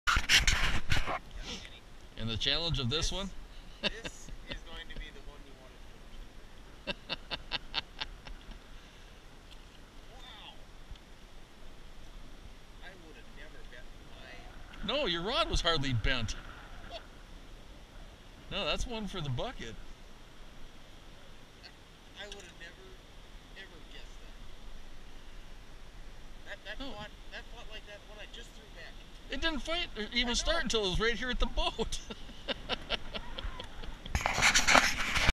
20180921 Trolling for Small Lake Crappie
Hot September action, lousy September weather